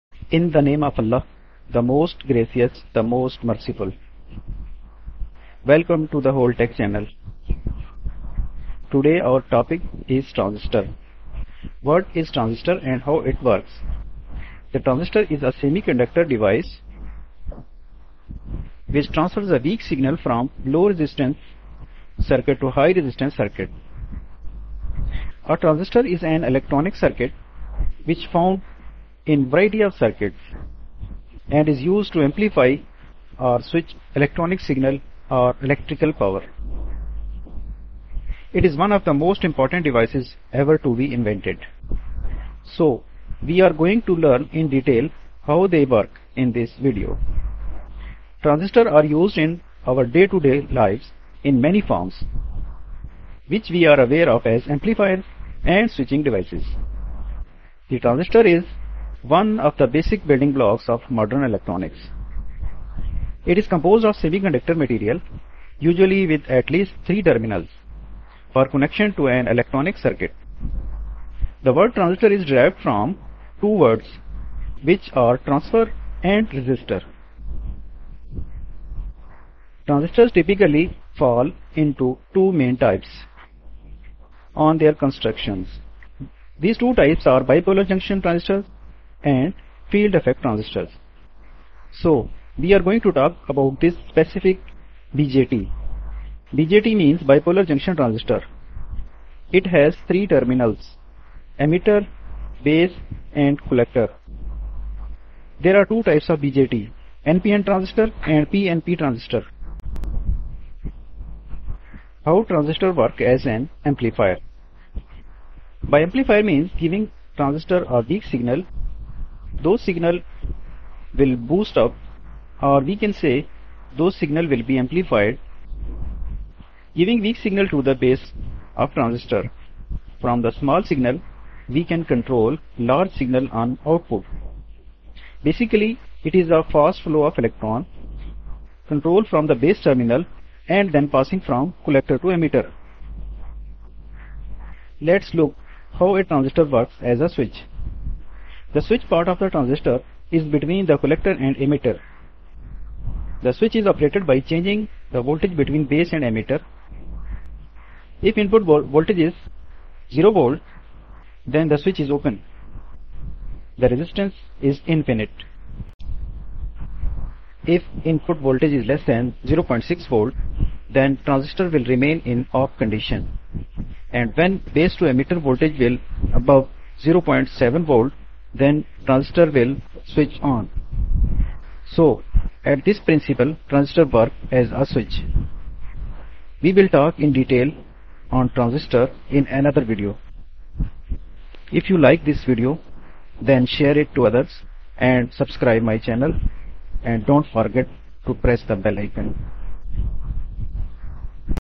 The Power Behind Your Electronics: The Transistor Explained
In this video, we'll be addressing the topic of Transistor - one of the most essential invention in science! We'll be explaining what Transistor is, what it is used for, and how you can supercharge your brain to understand this essential topic.

If you're looking to learn more about Transistor, or to improve your understanding of science concepts, this video is for you! By the end of this video, you'll have a better understanding of what Transistor is and how it works, and you'll be able to apply this knowledge to your own life!
transistors explained,transistor animation,transistor,transistor explained,how transistors work,what is a transistor,transistors introduction